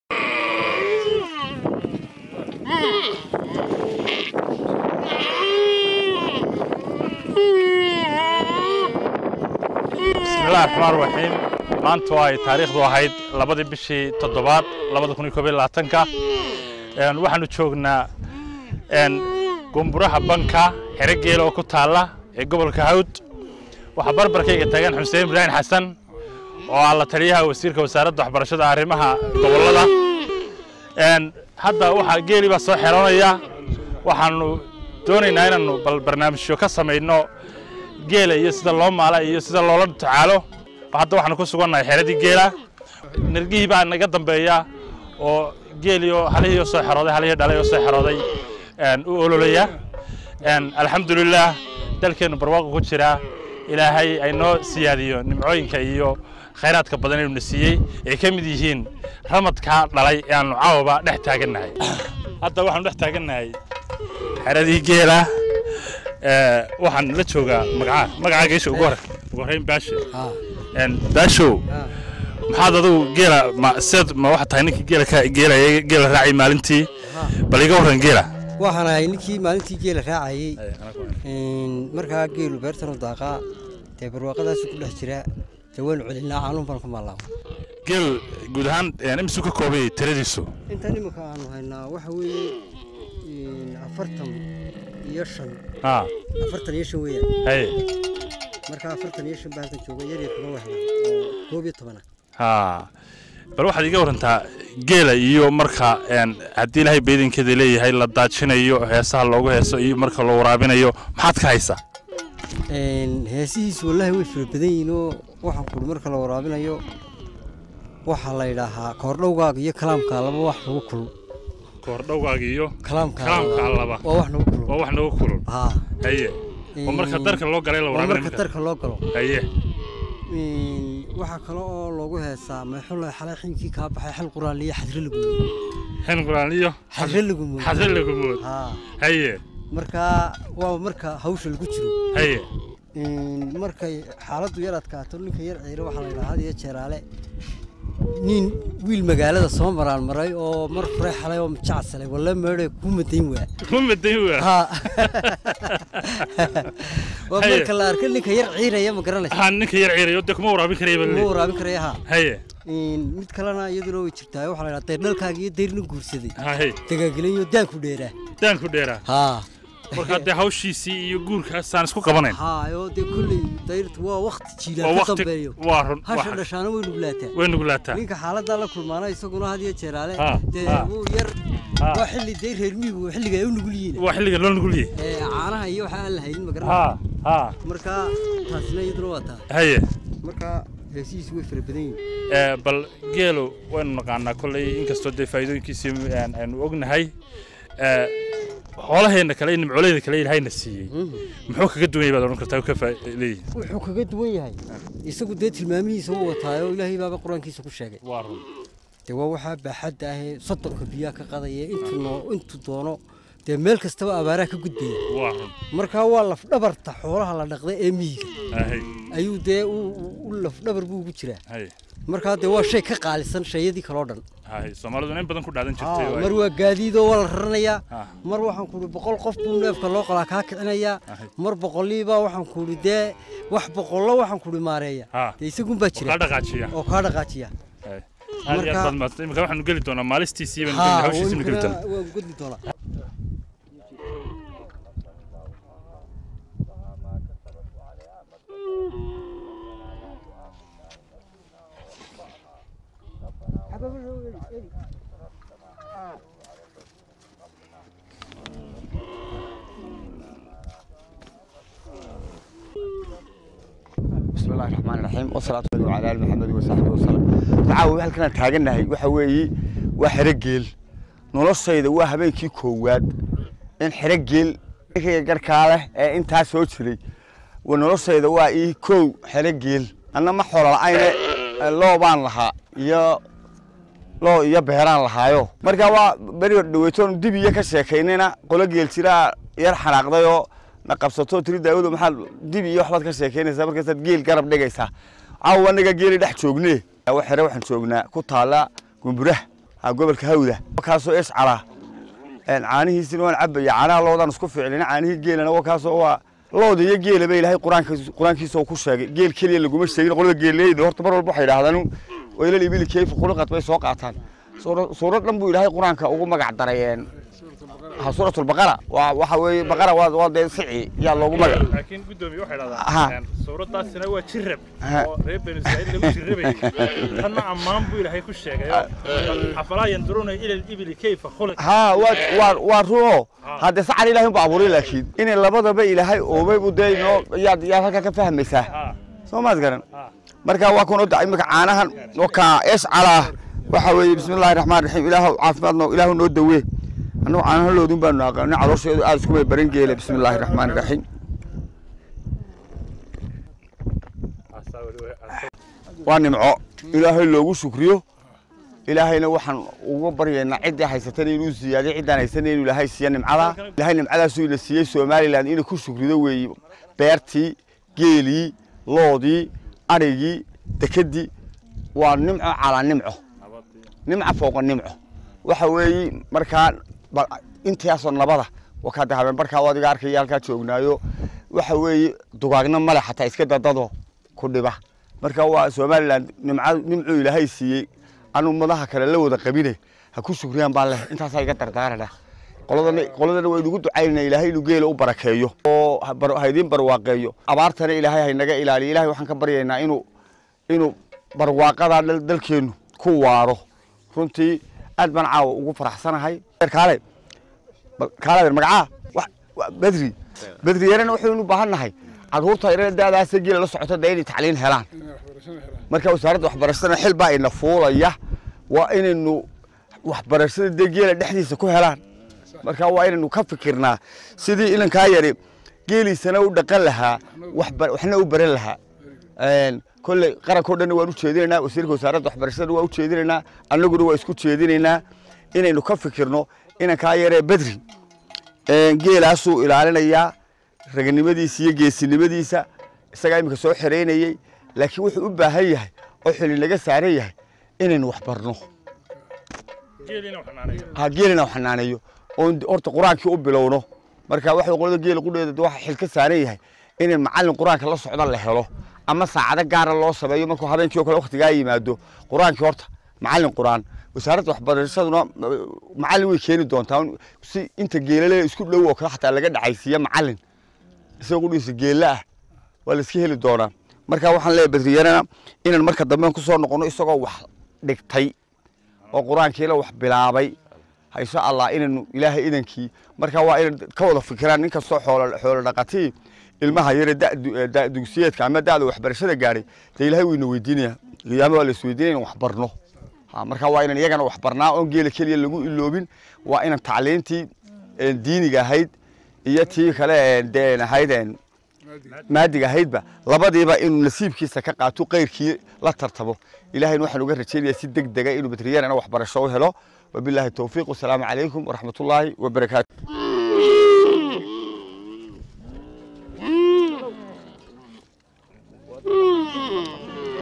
Duleedka Degmada Gunburaha & Booqashadii Galabta
Maxaad Ka Taqaanaa Heesaha Geela.

Qiniimada iyo kheyraadka dalkeena somaliland illaahay ku galladay maaha mid la soo koobi karo. Maanta oo ay taariikhdu ahayd 2/7/2021 ayaan maanta goor fiid-cawl ah waxan nasiib u helnay aniga iyo wefti uu hogaaminayey guddoomiyaha xafiiska imtixaanaadka qaranka JSL Daa'uud Axmed Faarax in aan soo booqano xero geel oo ku taala duleedka galbeed ee degmada Gunburaha banka oo ka tirsan gobolka Hawd. Waxan arrimahaa waraysigan kooban kaga duubay Baashe oo ka mid ah wiilasha geelaa xanaaneeya iyo guddoomiyaha xafiiska imtixaanaadka oo weftigayagaa hogaaminayey.